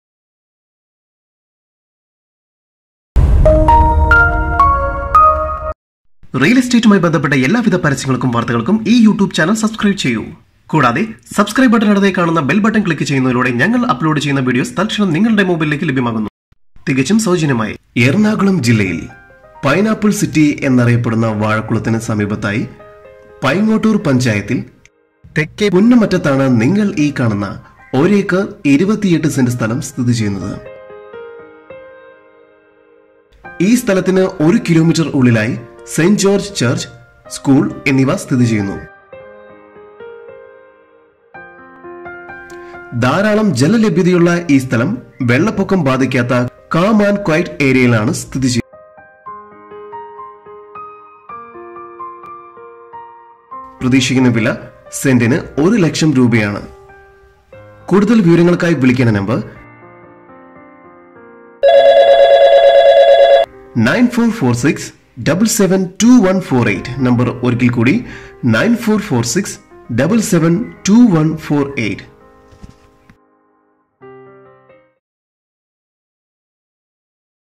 Land For Sale In Ernakulam | Vazhakullam | 2 Acre 28 Cent | Kerala Properties | +91 9446772148
Land  For Sale In Ernakulam
 Vazhakullam
 2 Acre 28 Cent land
price on property 1 lakhs per cent
For More Details
owner name : Mr Thomas

Published by

JUS MEDIAS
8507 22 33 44